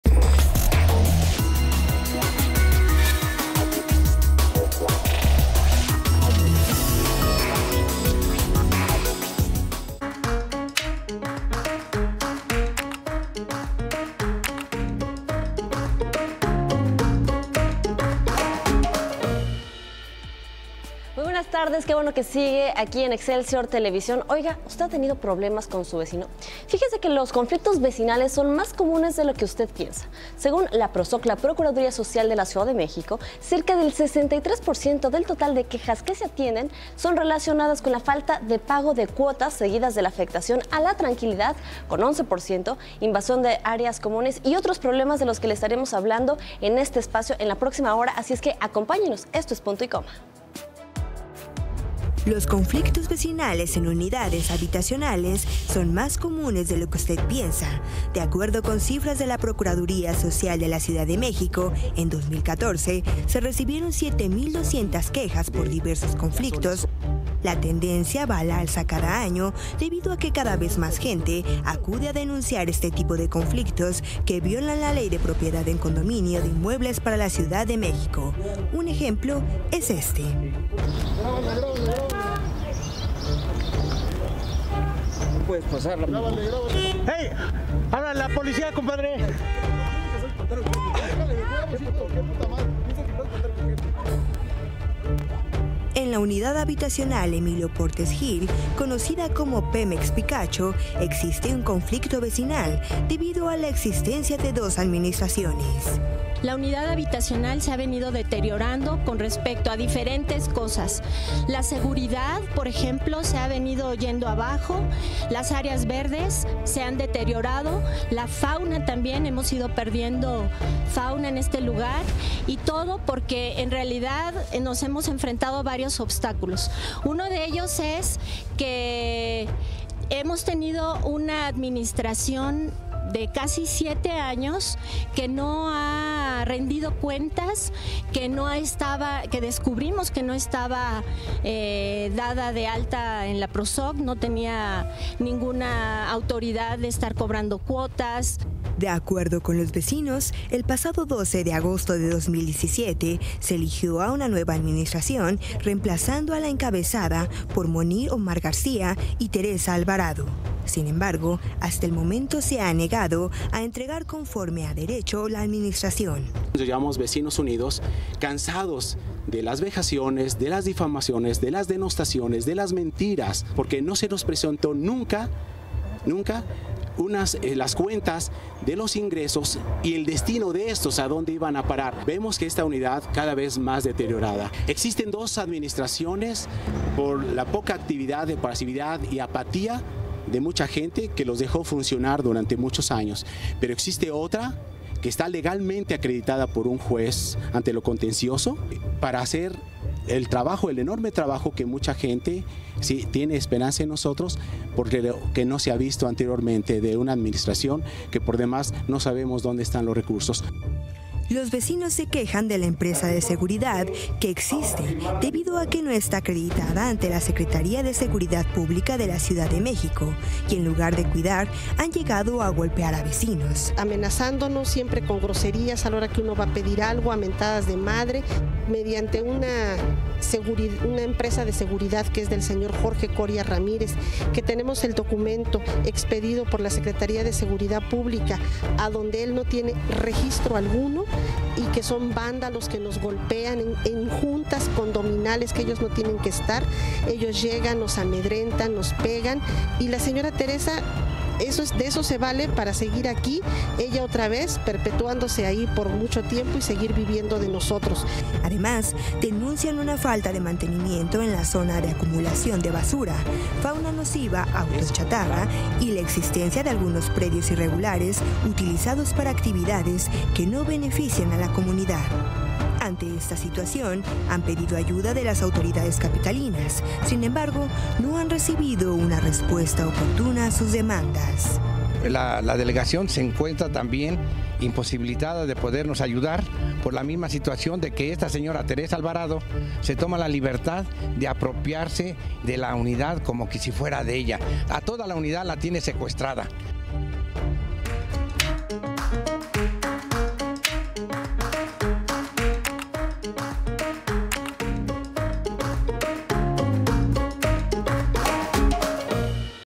Excélsior Tv Vecinos Unidad Habitacional Emilio Portes Gil tienen problemas fuertes
Video de Alejandro Arturo Guevara